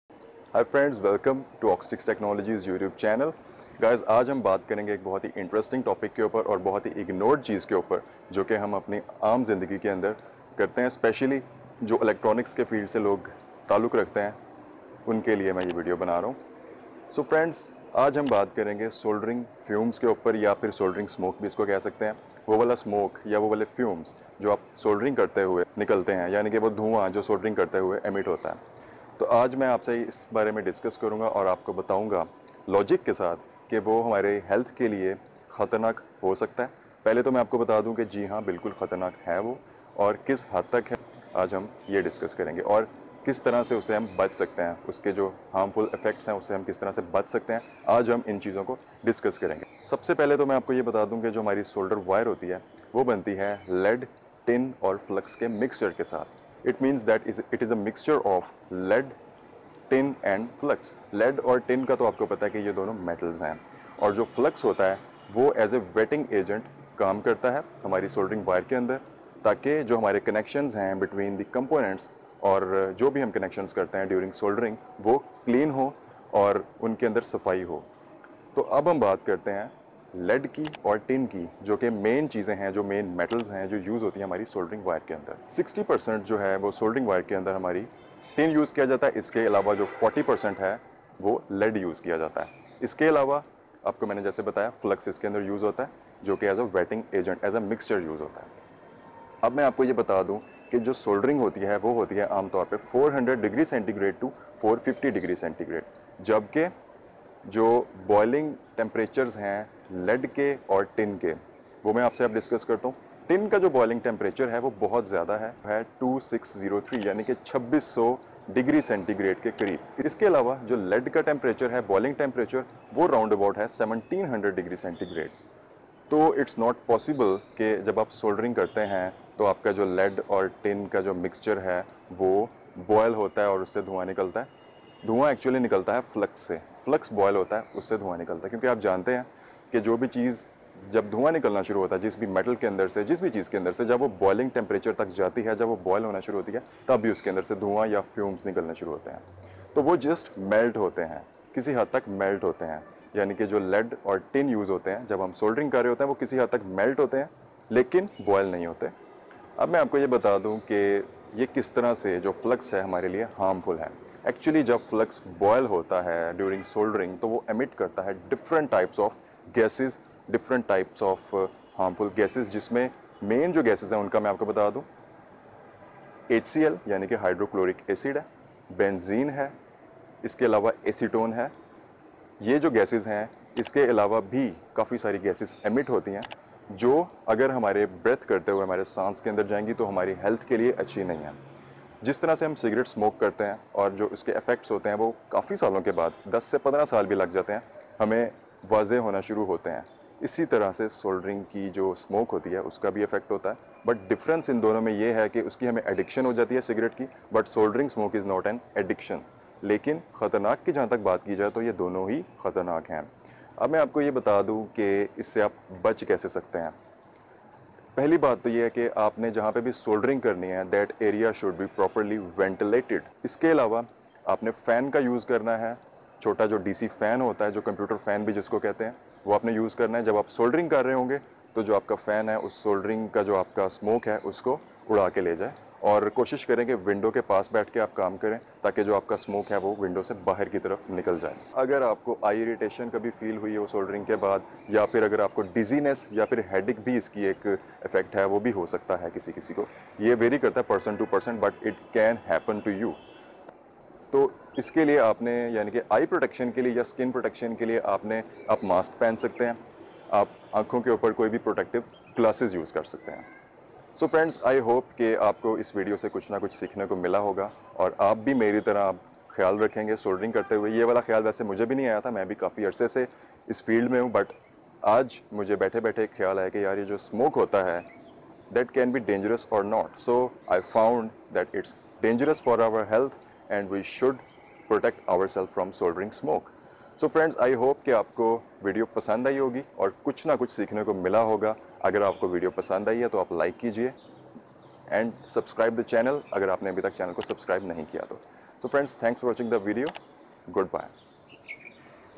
ARE SOLDERING FUMES BAD FOR HEALTH ? EXPLAINED !
Soldering Fumes are really bad for your health so please stay protected from the harmful effects flux emitted from the flux in the form of fumes.
This may effect your lungs, throat, eyes, skin in the long run and can cause dizziness, headache or the eyes irritation.
So use a proper fume extractor to extract the fumes out of your working area and try to wok in the ventilated environment.

PEACE OUT !